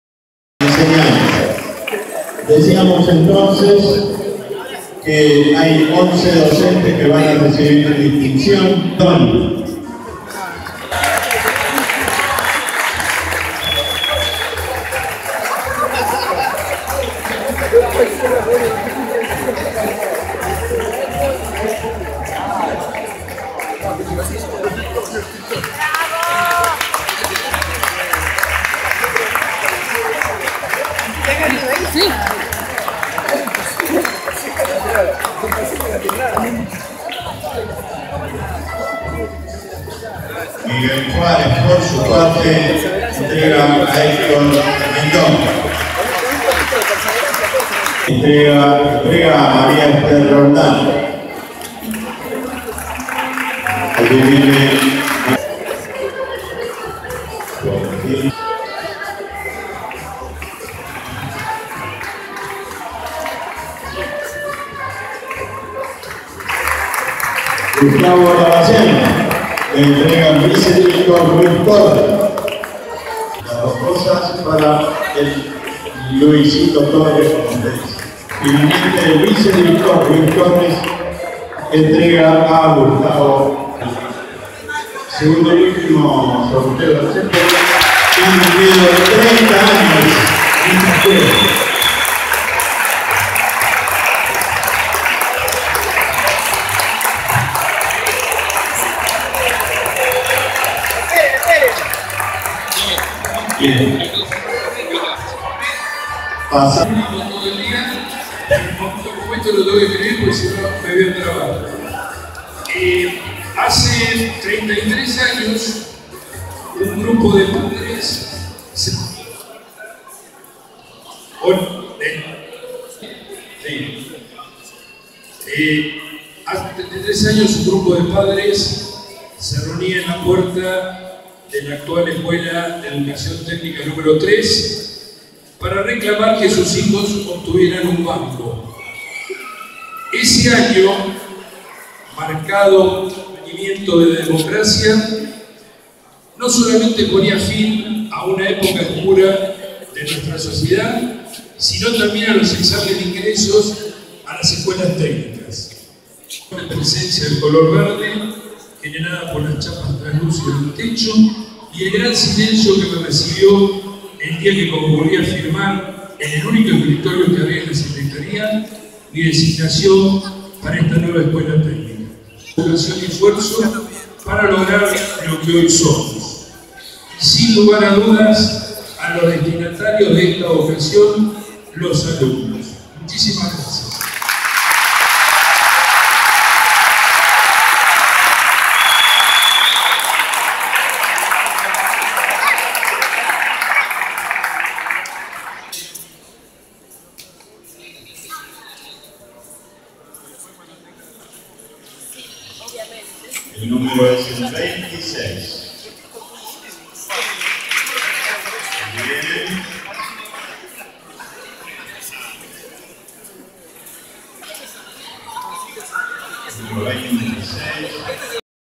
Los 30 de la 5 (4/4)
Entrega de medallas a los profesores que cumplieron 30 años de servicio en la Escuela de Educación Secundaria Técnica N°5 "Amancio  Williams" en sus 30 años de vida.